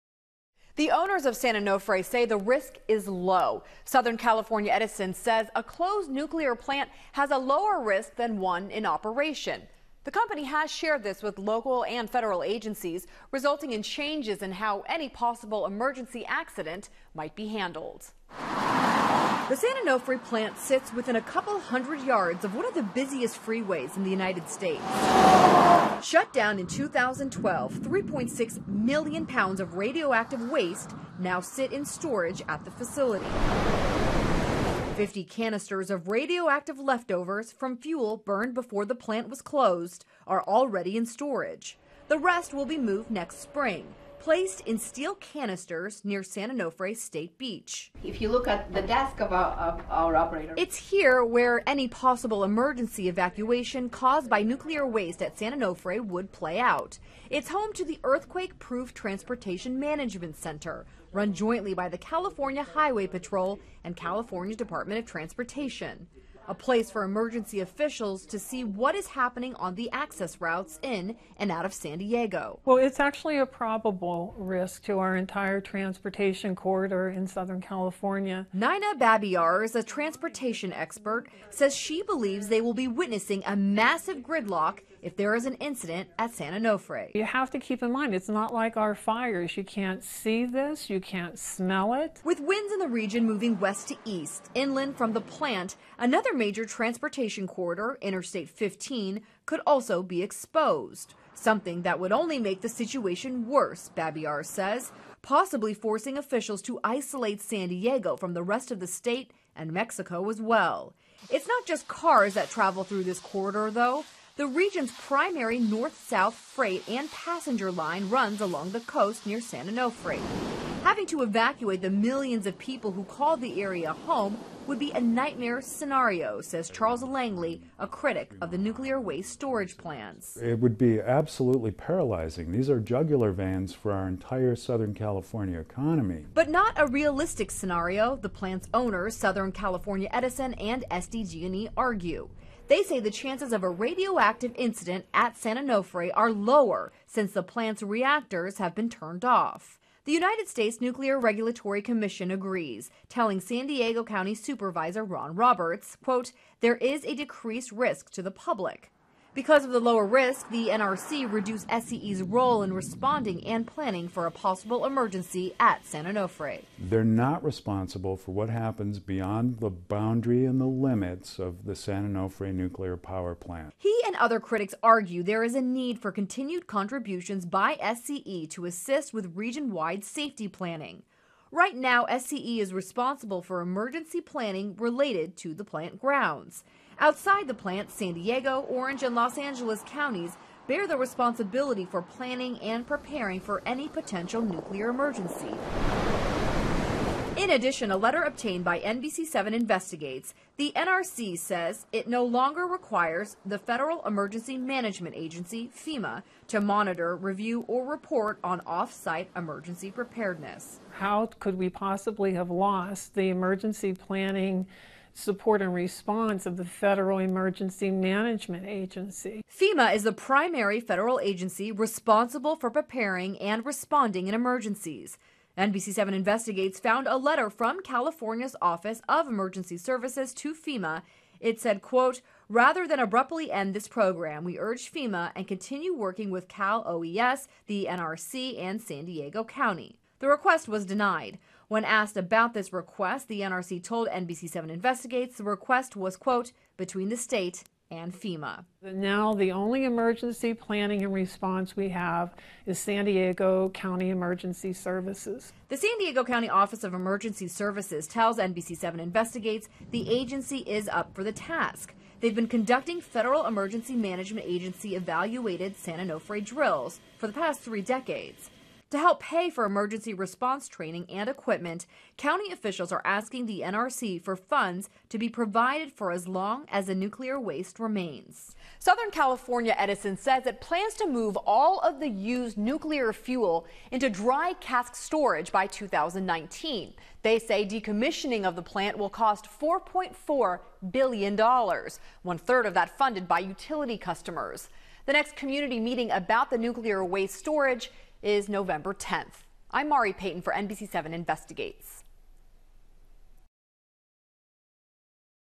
Part 2: Critics Question Plans For Nuclear Waste Storage At San Onofre
Published for archival purposes under